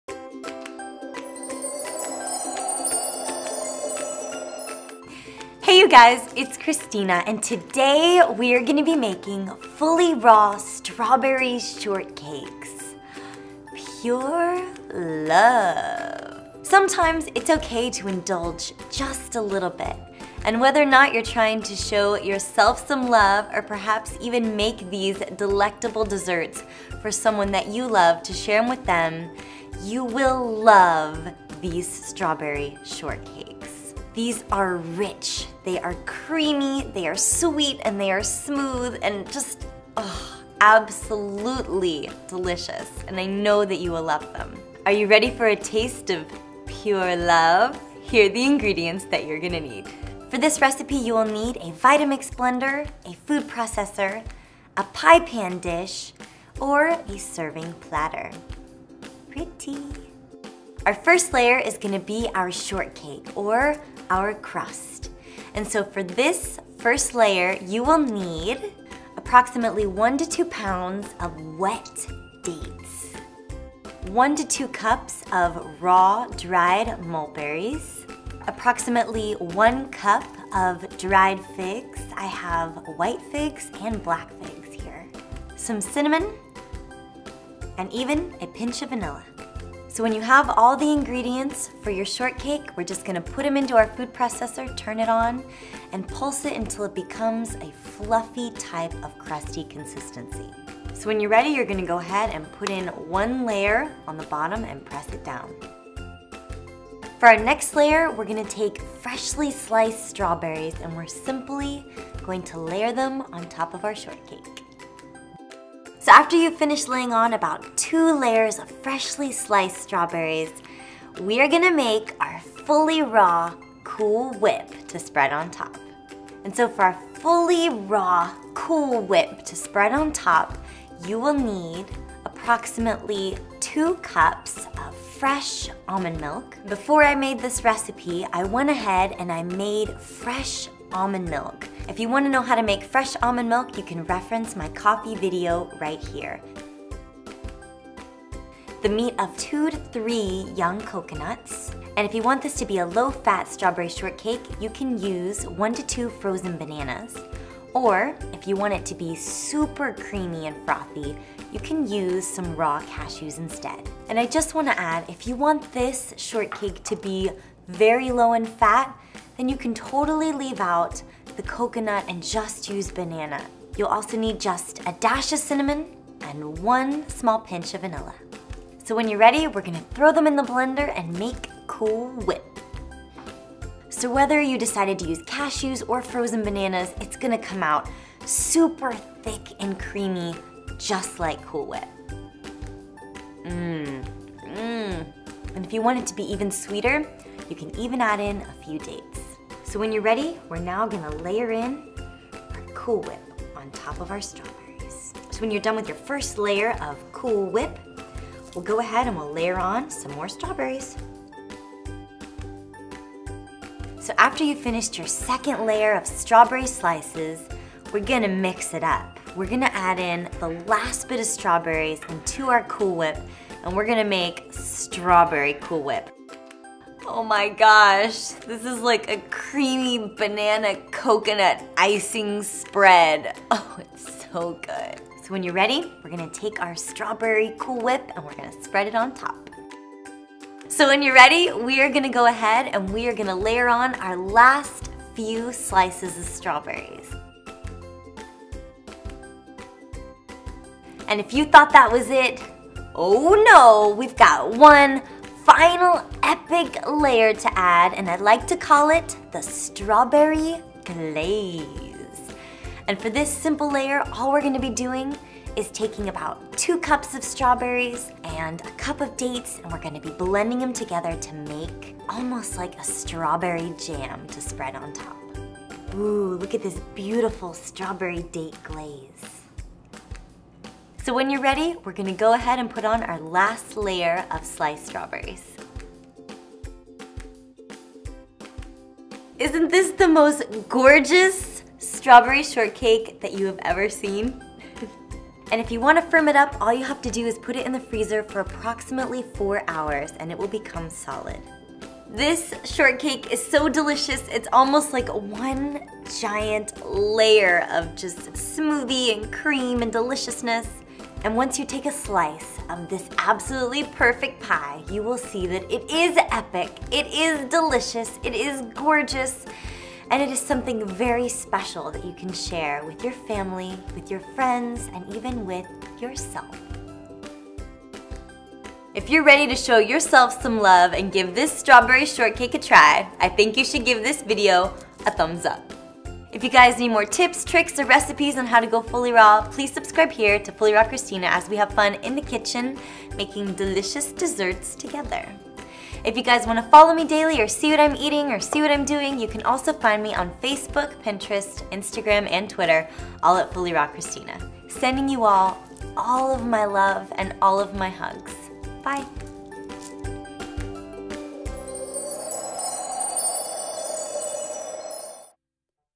FullyRaw Strawberry Shortcake!
Easy Raw vegan strawberry shortcake! The perfect dessert to make for your loved ones or even yourself! 7 layers of sweet, fluffy heaven! BE MINE?!

 A special Valentine's Cake to make for your loved ones or even for yourself! This recipe of EPIC MEAL TIME proportions will win the heart of ANYONE!

 Ingredients for the crust:
 2-3 lbs of pitted, fresh dates
 1-2 cups of dried, raw mulberries
 1-2 cups of raw, dried figs (white or black)
 1 pinky of vanilla bean
 1 tbs of cinnamon

 Run all ingredients through a food processor until the mixture reaches a crust-like consistency. Push the crust into the bottom of a pie pan or a cake dish, and get ready for the next layer! If you want your cake to slice smoother, feel free to add wax paper lining on the bottom of the cake.

 Add a sliced layer of strawberries onto your crust.

 FullyRaw Cool Whip Ingredients:
 2 cups of freshly prepared almond milk
 Either 1-2 frozen bananas or a half cup of raw cashews
 The meat of 2-3 young coconuts
 Note: if you want the cake to be lower in fat, use the bananas instead of the cashews and replace the almond milk with coconut water.

 Blend all ingredients in a high speed blender like a Vitamix. Spread this FullyRaw "Cool Whip" onto your first layer of strawberries. Add a second layer of sliced strawberries on top and get ready for another layer of Cool Whip!

 For your FullyRaw Strawberry Cool Whip, simply use the remaining white cool whip and blend it with a few strawberries to turn it pink! Spread this layer on top of your second layer of sliced strawberries and top it with ANOTHER layer of sliced strawberries!

 Ingredients for the Strawberry Glaze:
 Half to one pound of fresh strawberries
 Half a cup of fresh, pitted dates

 Blend the ingredients for the strawberry glaze and spread it on top! Add your final layer of sliced strawberries! Place your cake into the freezer for approximately 4 hours to harden it. When ready, take it out, slice it up, and ENJOY! Share with family and friends!

 I hope that you love LOVE LOVE this FullyRaw Strawberry Shortcake! HAPPY VALENTINE'S DAY! :)

❤ Have a meaningful transformation with these FullyRaw Vegan Challenges ❤

Also, JOIN me in my upcoming retreats!